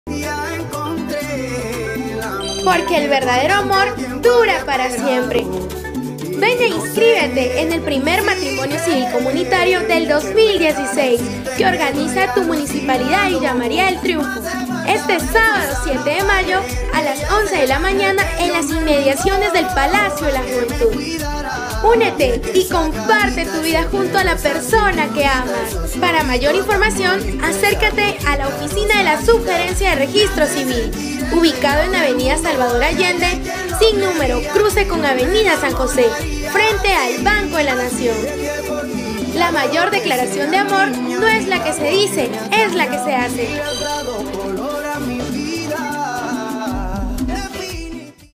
SPOT MATRIMONIO CIVIL 2016
Ya están abiertas las inscripciones para el 1er Matrimonio Civil Comunitario 2016, que organiza la Municipalidad de Villa María del Triunfo. este Sábado 7 de Mayo a las 11.00 am. Para mayor información acércate a las instalaciones de la Sugerencia de Registro Civil .